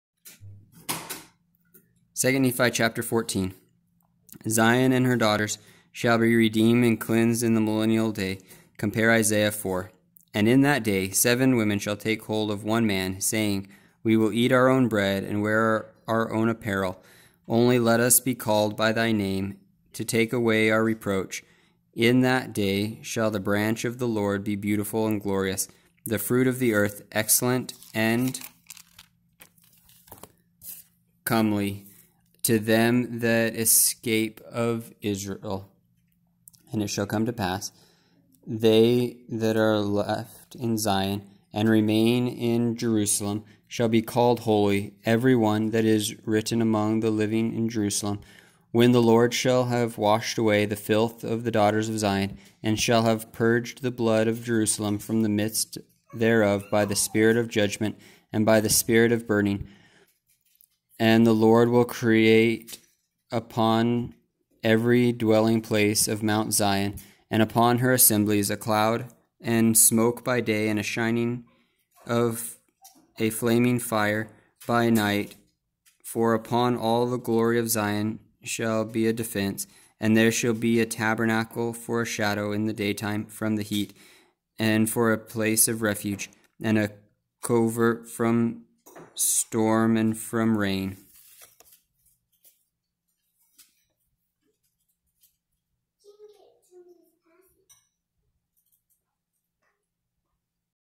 The Book of Mormon - 2 Nephi 14
2 Nephi 14 read aloud from The Book of Mormon: Another Testament of Jesus Christ. The Book of Mormon read aloud one chapter at a time. Study aid for those that don't have the time to read themselves. Follow along for help understanding scriptural pronunciations. Perfect for those learning how to read English, there is power in the scriptures that will increase your ability to learn to read and understand English.

A note on the markings: References to God the Father or Jesus Christ are highlighted in red. References to the Holy Ghost are highlighted in blue. The quoted words of God the Father, Jesus Christ, or the Holy Ghost are underlined in blue pen. This chapter contains 3 references to the Godhead.